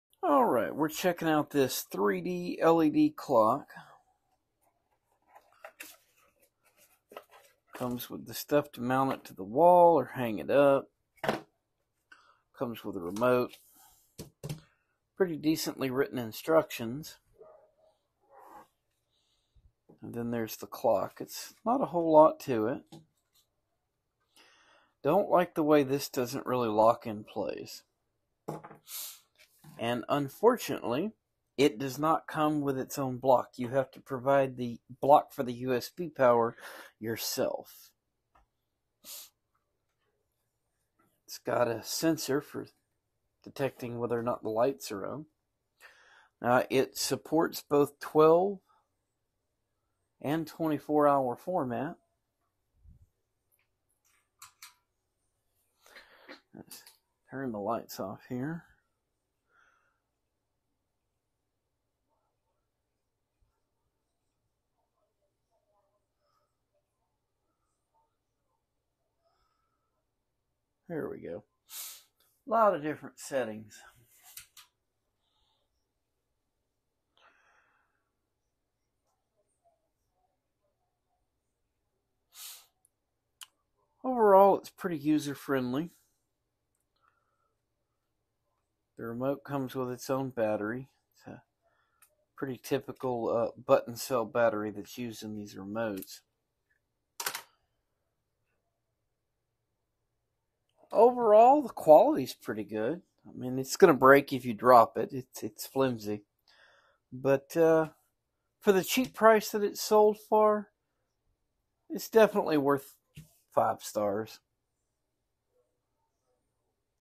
Review - COVERY 3D LED Digital Clock
A short review of COVERY 3D LED Digital Clock sold on Amazon